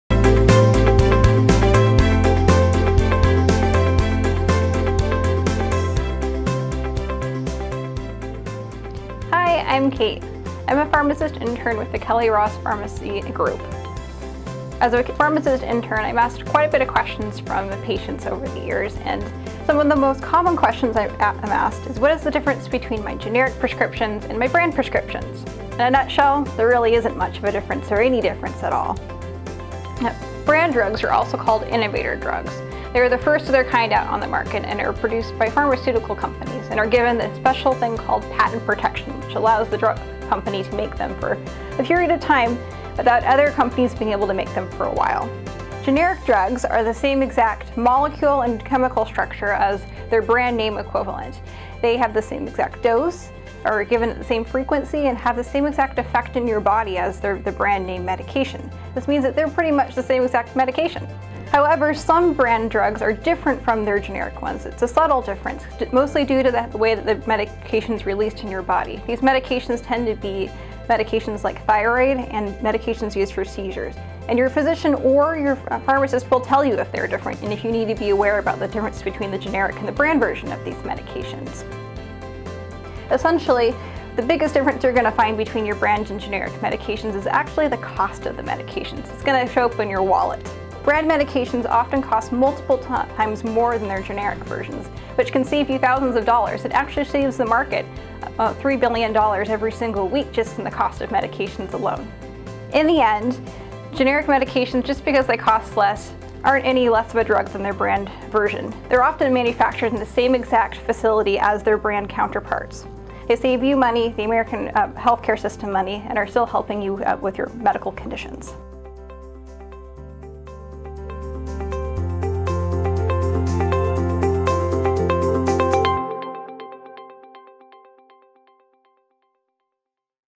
Generics vs Brand Name
As pharmacist, we are often asked if there is any major difference between a generic drug verses a brand name equivalent. Today, we answer that question as part of our on going Ask The Pharmacist series.